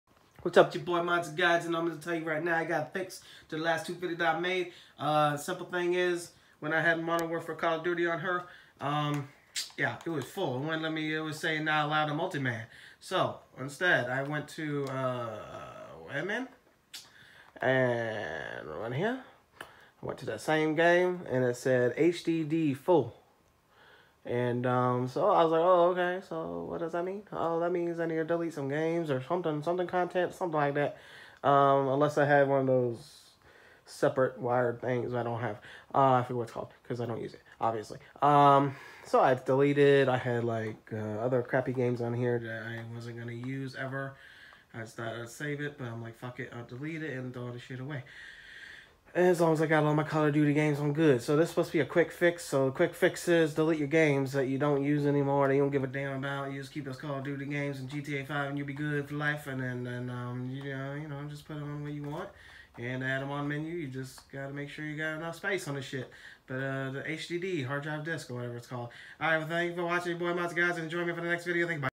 Cd to HDD “copy not aloud” quick fix ps3 multi-man/webman
Cd to HDD “copy not ￼Allowed￼ error” fix

It’s simple delete games you no longer want or need or buy a bigger HDD

Love making videos even if it makes me sound like a ass